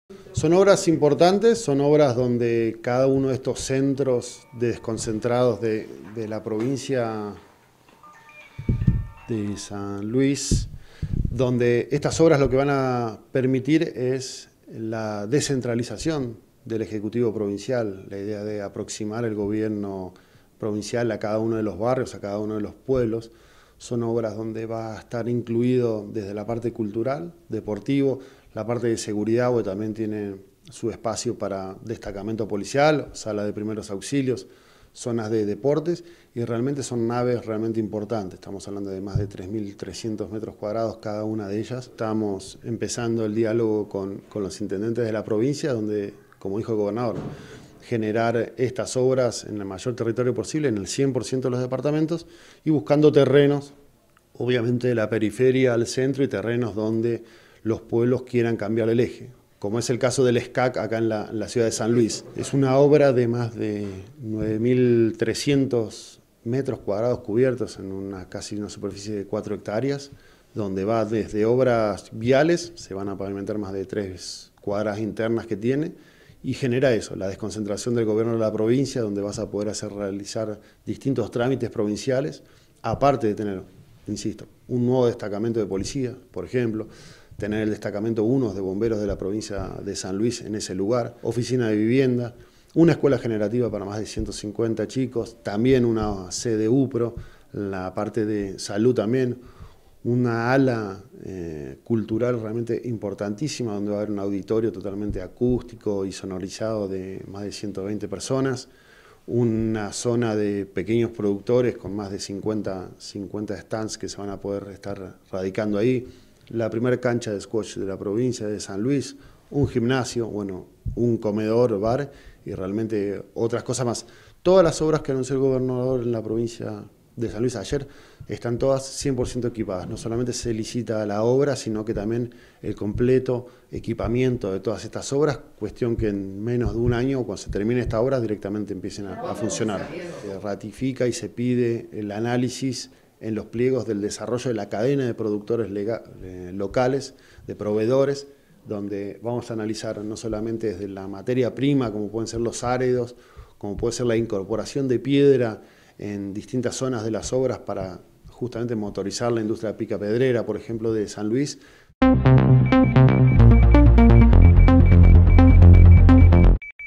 30-09-2019///ALCANCE DE LAS OBRAS QUE ANUNCIO EL GOBERNDOR
Felipe Tomasevich - Mtro Infraestructura
Tras los anuncios del gobernador el pasado viernes el ministro de infraestructura visitó el senado provincial para explicar el alcance de las obras que se hará en todo San Luis. El proyecto de los viernes es descentralizar la administración pública y por eso quería que las islas de oficinas en distintos puntos de la provincia